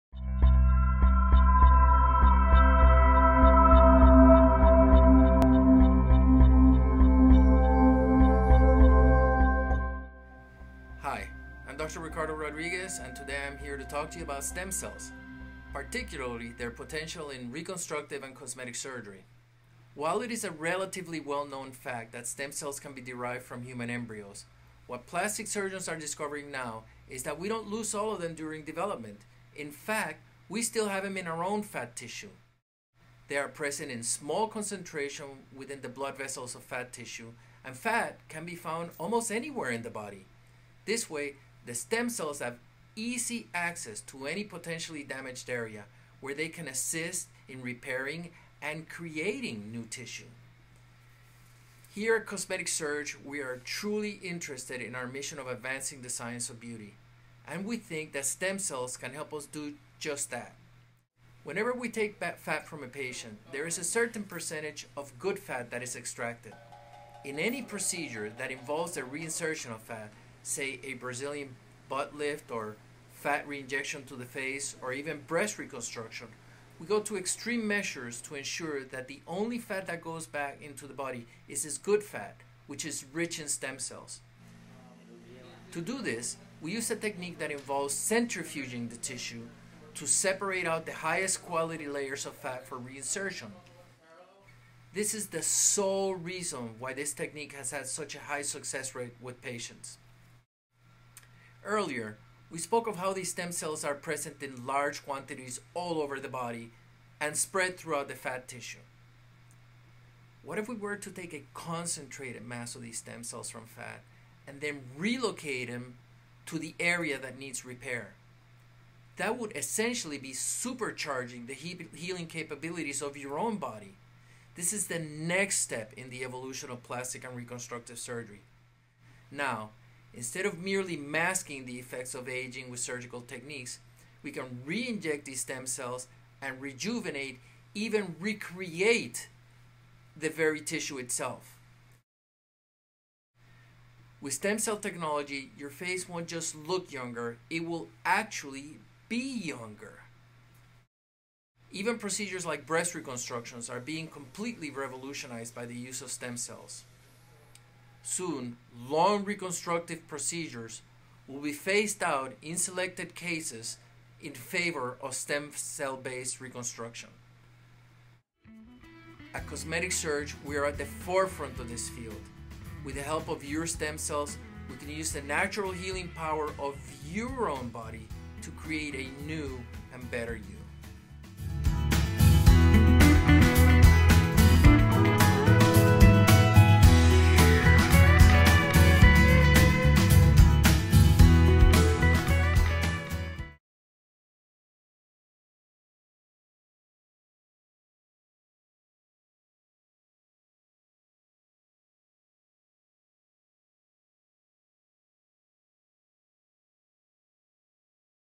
Stem Cells from Liposuction Fat: Plastic Surgery's New Frontier
Plastic Surgeons have been performing fat grafting / fat injections for over a decade . The technique for fat processing has evolved such that properly processed fat is rich in stem cells.  Plastic surgeons extract unwanted fat, process it to get the 'good fat' (stem cell rich), and reinject it into areas to restore youth.

The 'good fat' is your own stem cell enriched cells! This technology is available now and being performed by experienced plastic surgeons to restore youth to the face, breast, buttocks, and hands.

Dr. Ricardo L Rodriguez MD
1300 Bellona Ave
Lutherville-Timonium, MD 21093